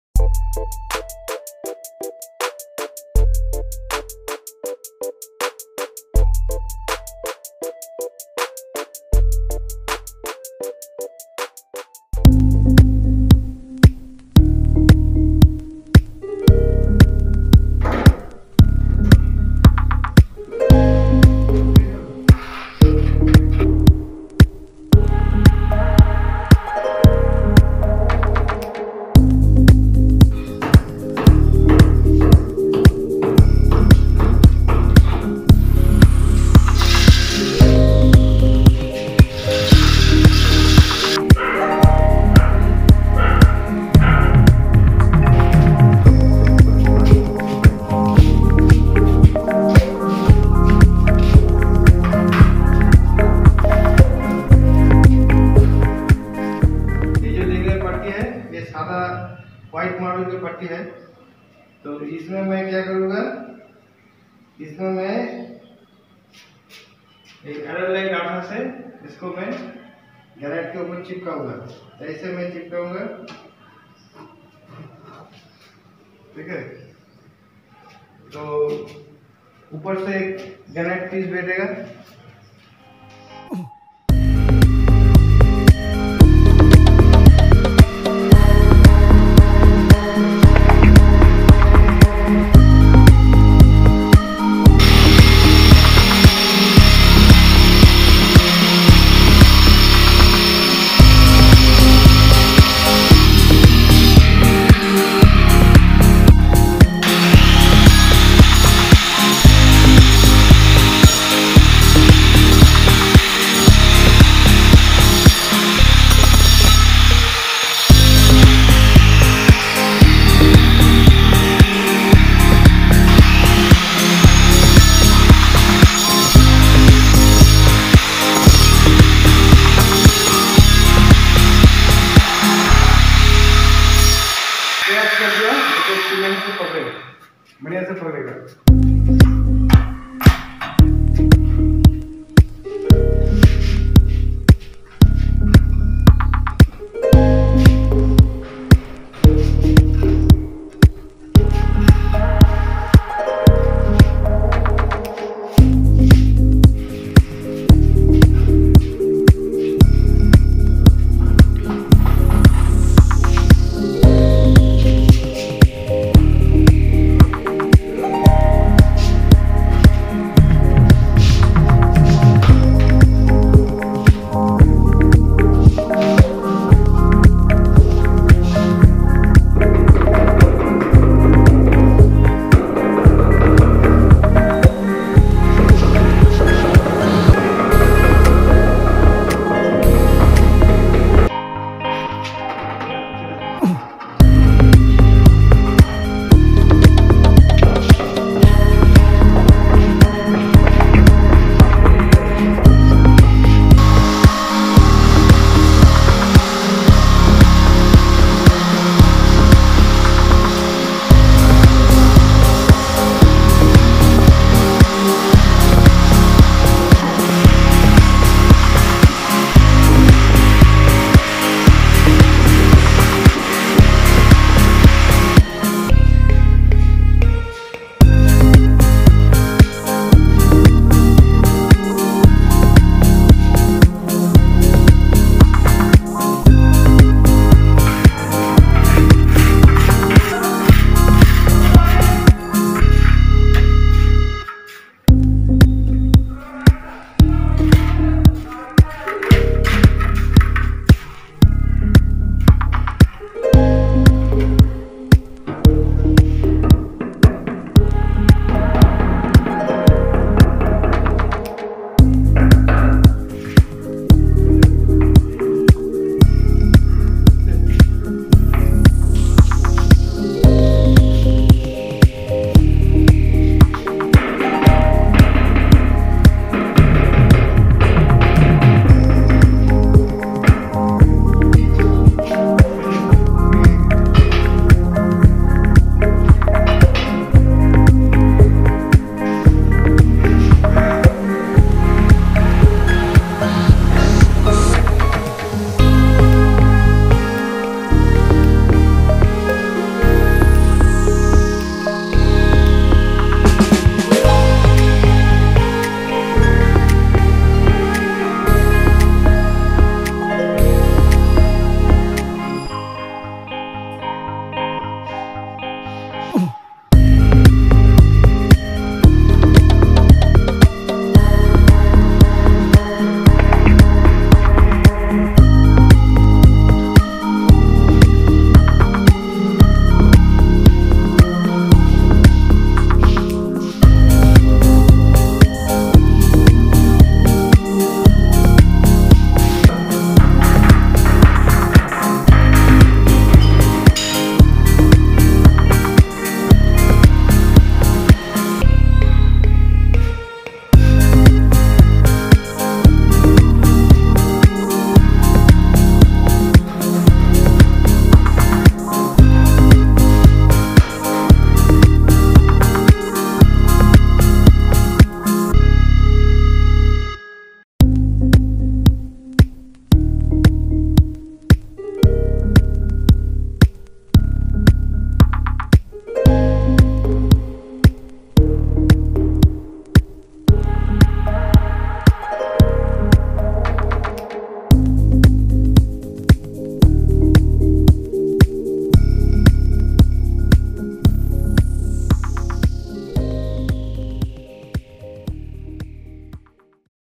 How To Install Granit = kitchen working table design | Dilip Art
Hello friends, I am Dilip Sarkar. Today I will show you how to make a double border with granite in Kitchen .